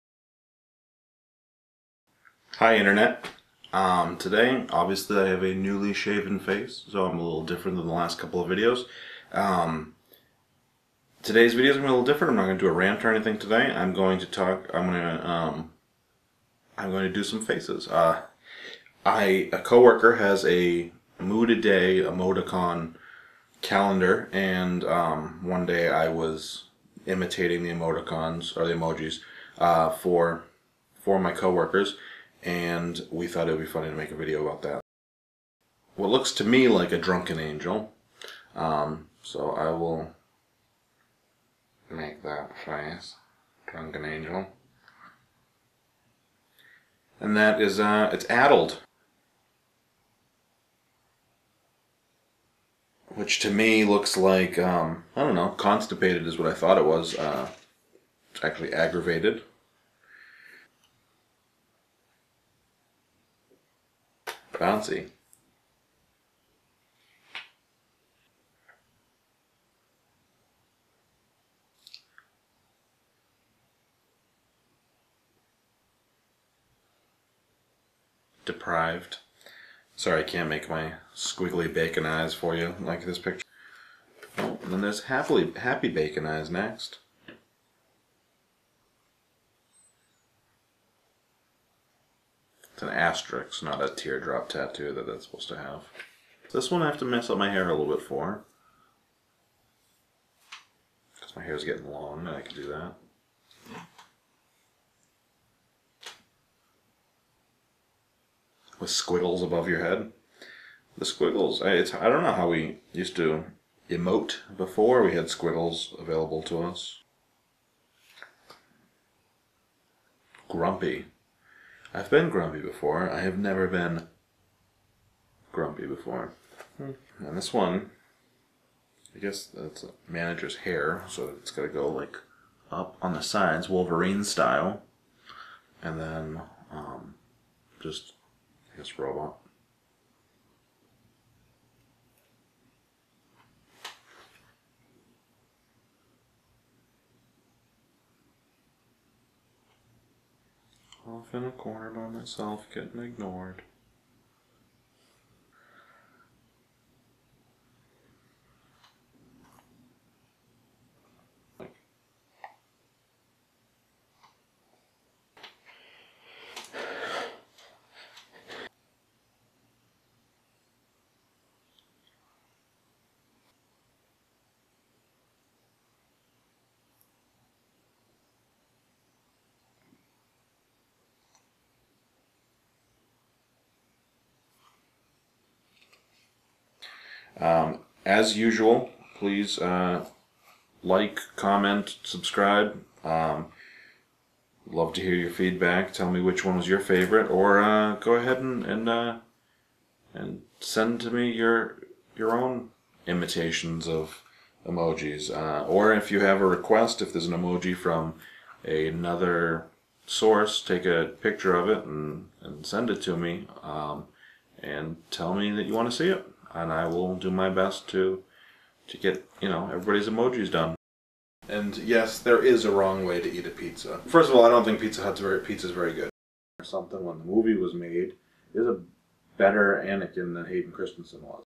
Imitating Stupid Emojis
In this video, I demonstrate my useless talent of immitating emojis, or as other people might refer to it, actually showing emotion like a human.  Enjoy!